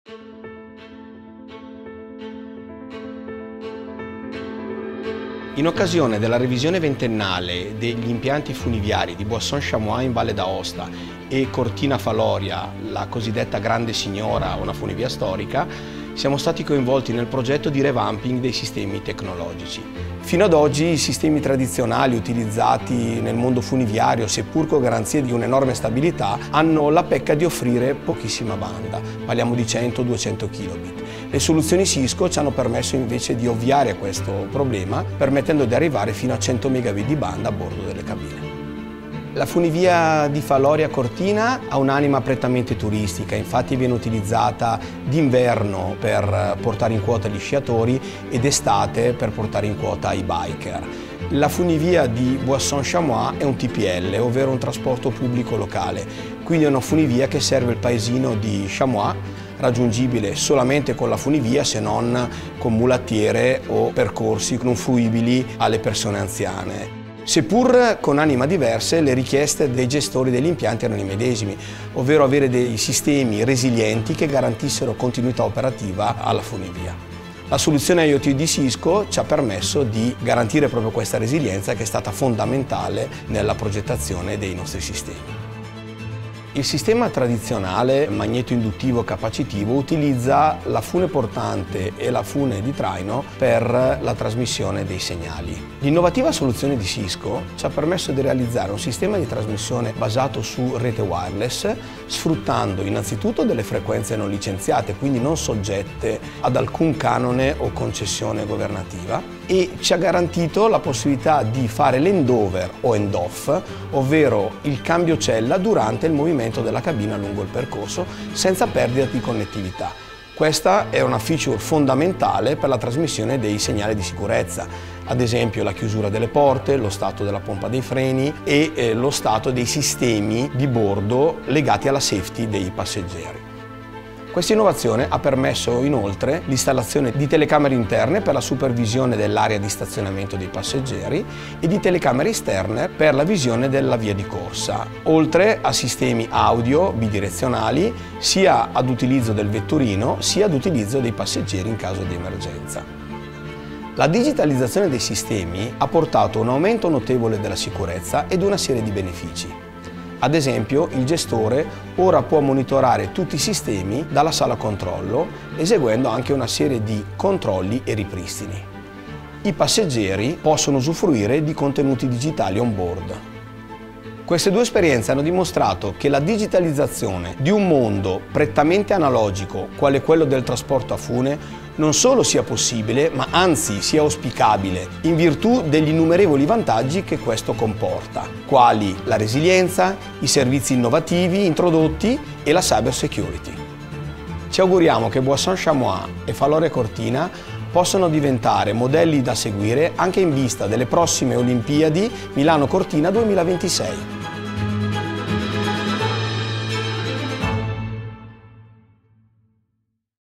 CISCO: Innovazione ad alta quota
Cisco, insieme a Security Trust, ha lavorato per rendere le funivie di Buisson-Chamois e Faloria più resilienti e sicure, offrendo una migliore esperienza ai passeggeri.